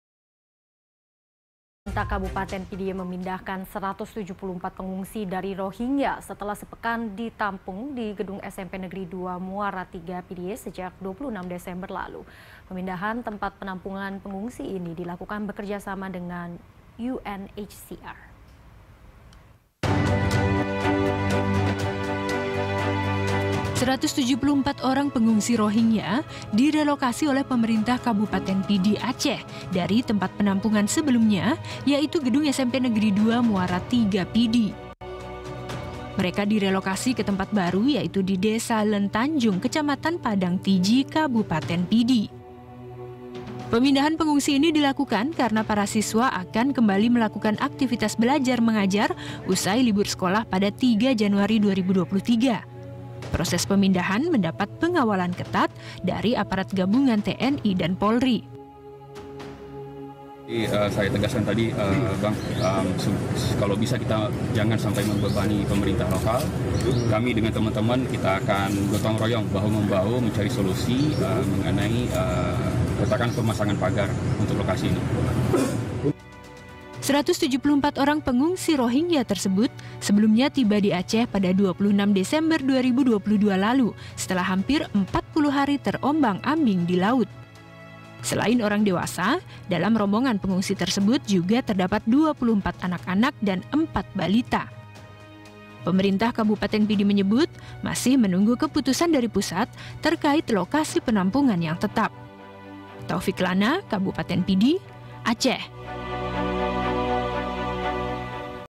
Pemkab Pidie Bersama UNHCR Relokasi 174 Pengungsi Rohingya
Pemkab Pidie memindahkan 174 pengungsi Rohingya setelah sepekan ditampung di Gedung SMPN 2 Muara Tiga Pidie. Pemindahan dilakukan bekerja sama dengan UNHCR.